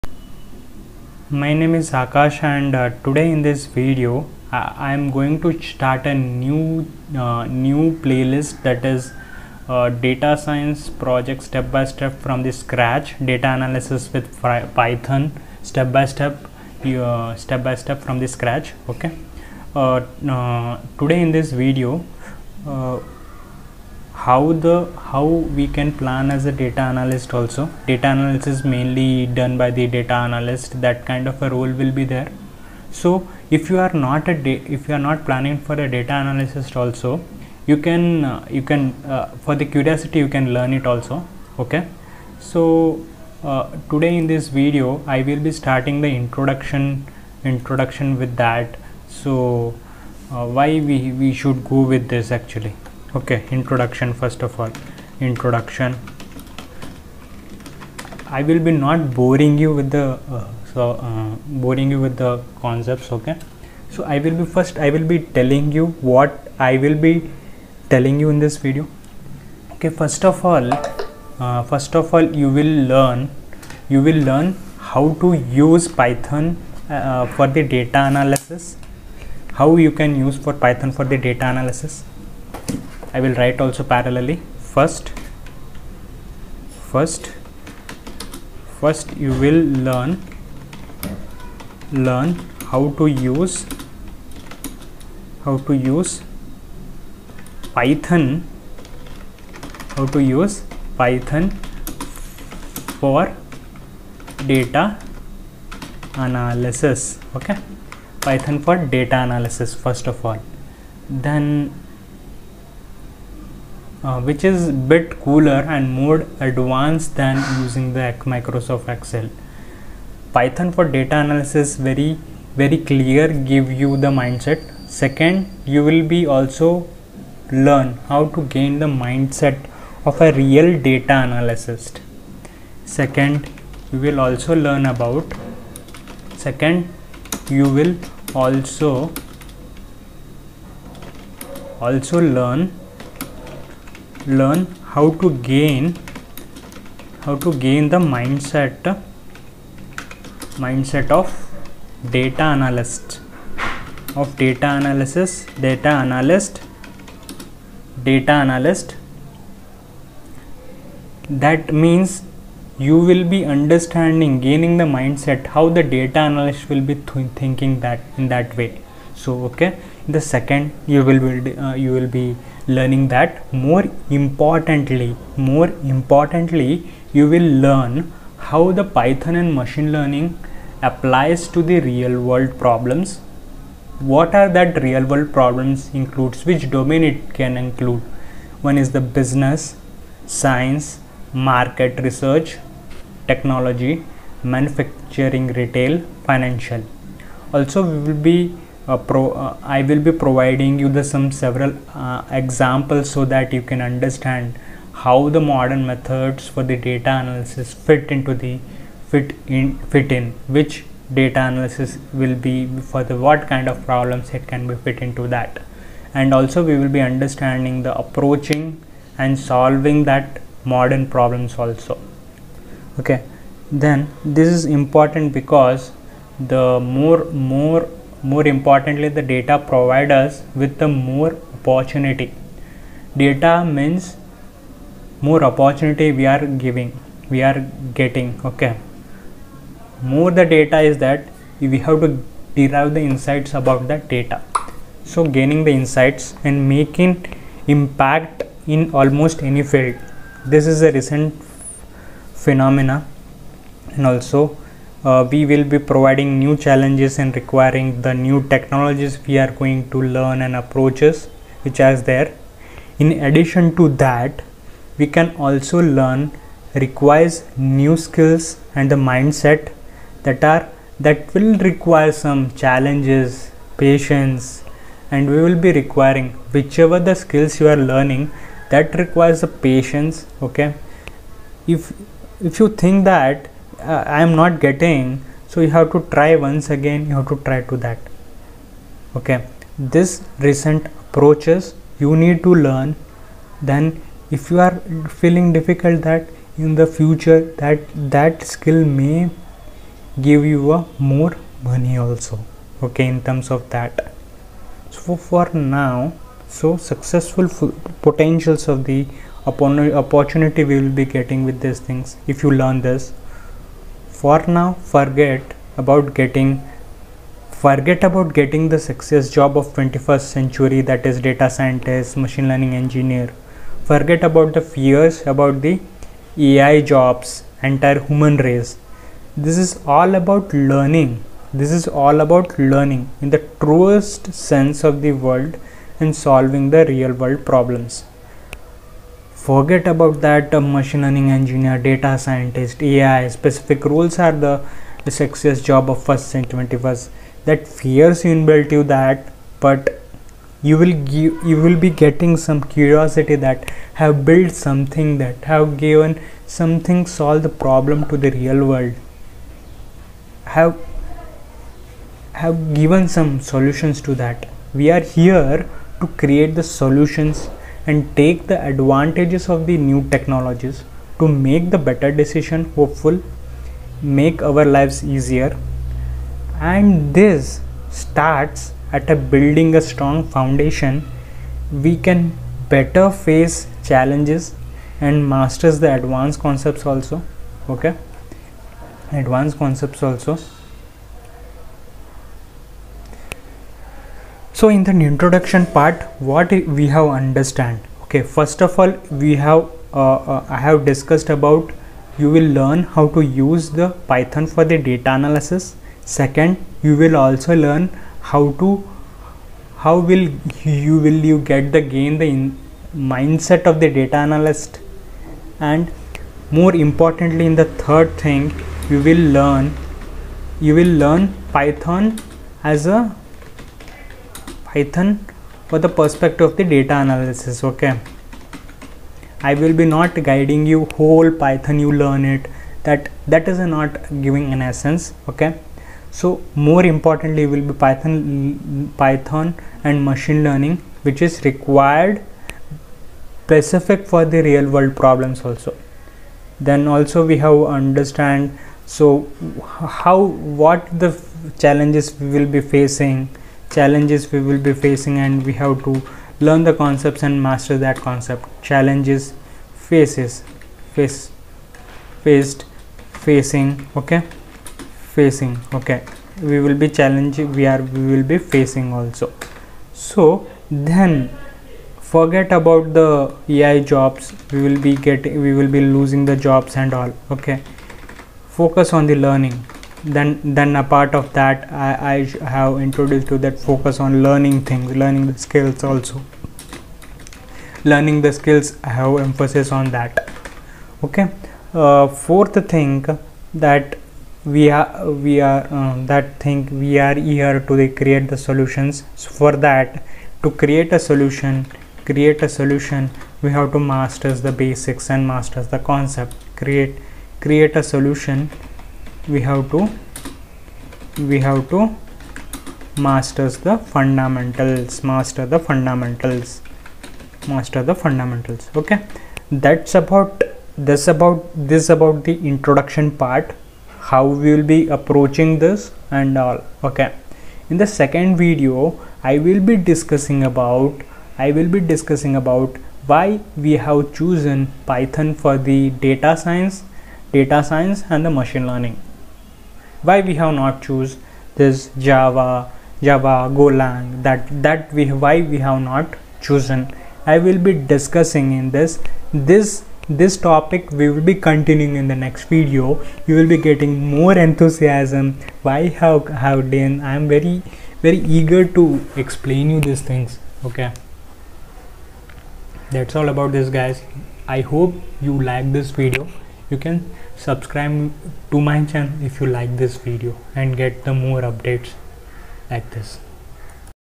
Introduction to python for data-analysis
In this Video Discussed about the How can you think as dataAnalyst and also about  skillls required for DataScience,AI, ML